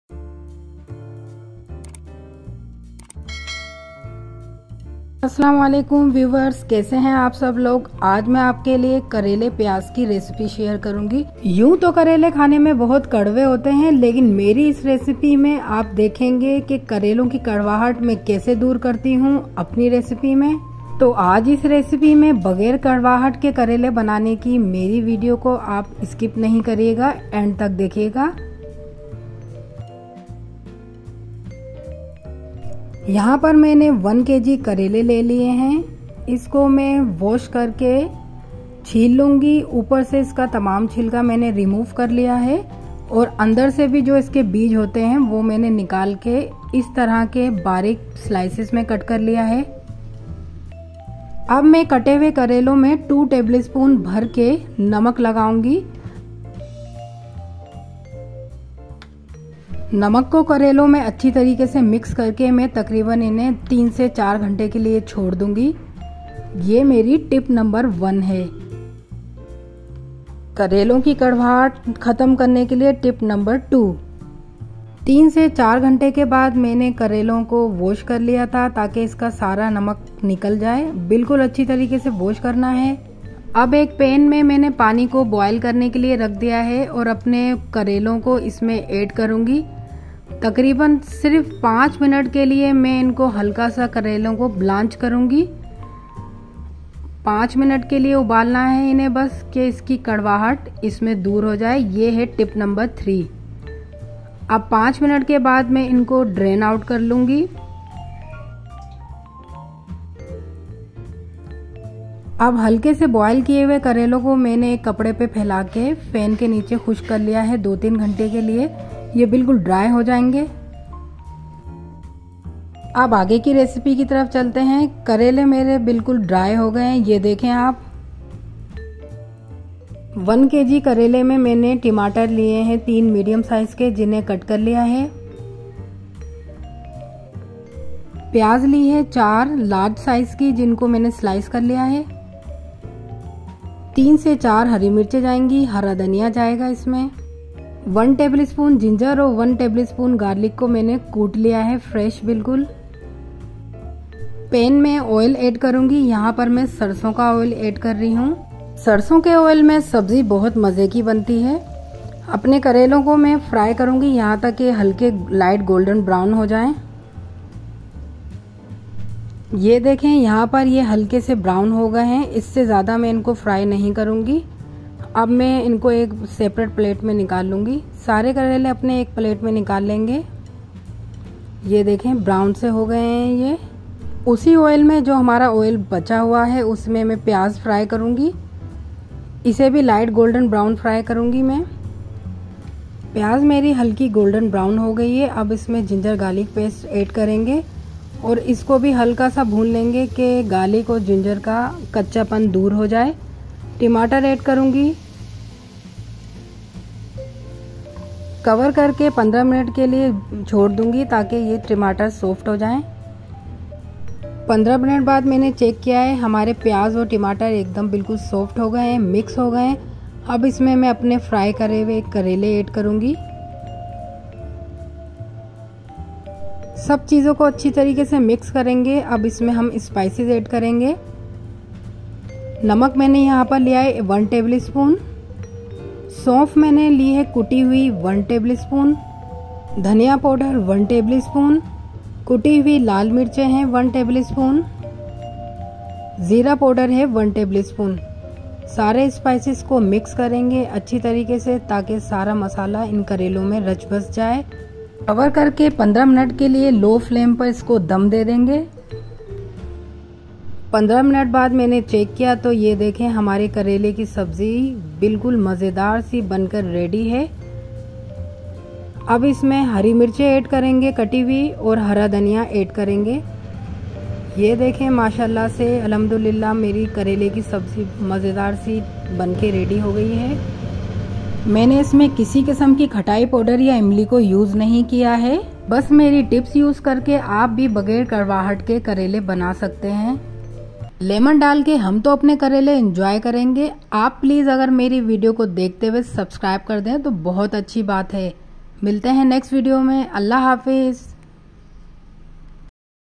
Karela piaz recipe |کریلا پیاز بغیر کڑوا ہٹ کے |Bitter gourd with onion recipe |crispy Karela piaz.
A quick and simple recipe for you .You will always get it right with this recipe.
. کریلا کی کڑواہٹ ختم کرنے کا طریقہ
نہایت مزیدار کریلا پیاز ترکیب
Karela piaz recipe|Bitter gourd with onion recipe
ingredients,

Bitter gourd ( Karela) 1 kg
onion ( Piaz)4 Large size
Tomato ( Tamater)3 medium size
green chillies 4 to 5
coriander leaves

Ingredients for spices,
Salt to taste
cumin powder ( Zeera)1 Tbsp
Saunf crushed 1 Tbsp
Red chili flakes( Kuti Mirch) 1 Tbsp
coriander powder (Dhania) 1 Tbsp
oil (Sarso oil)1 cup

Thanks.